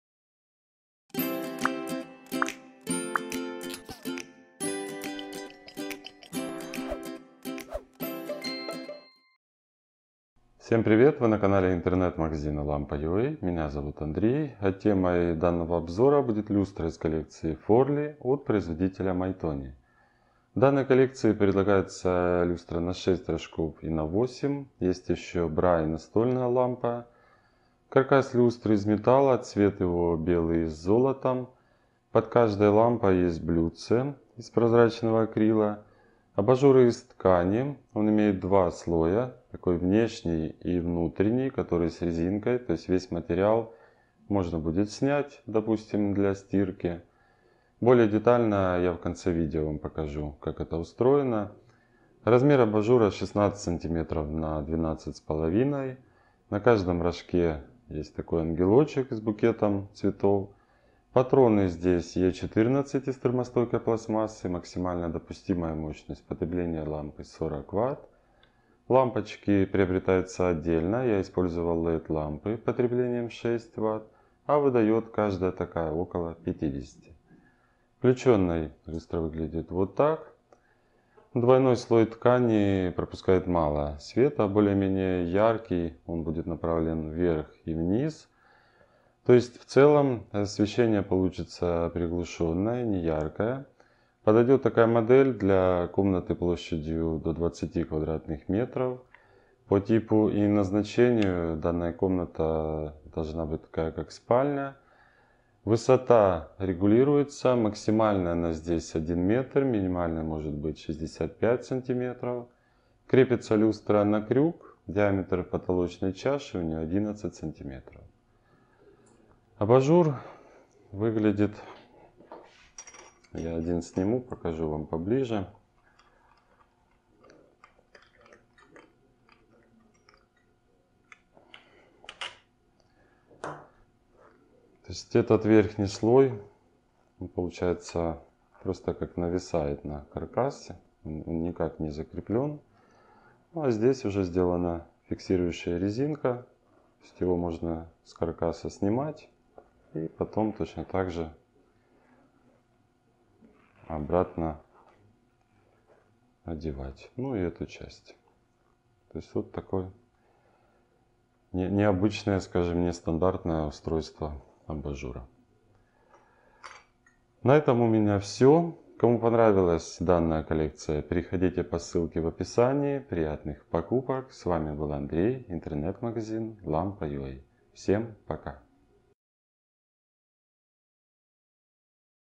Люстра MAYTONI 92244 (MAYTONI Forli ARM394-06-WG) обзор
Купить люстру с абажурами в стиле прованс MAYTONI 92244 (MAYTONI Forli ARM394-06-WG)

Обзор белой люстры с абажурами в стиле прованс из колекции Forli бренда MAYTONI (Германия)

 Из ролика вы узнаете про:
 - другие светильники из коллекции;
 - тип патрона;
 - материалы светильника;
 - технические характеристики;
 - максимальную мощность потребления светильника;
 - рассеивание освещения;
 - диаметр крепления;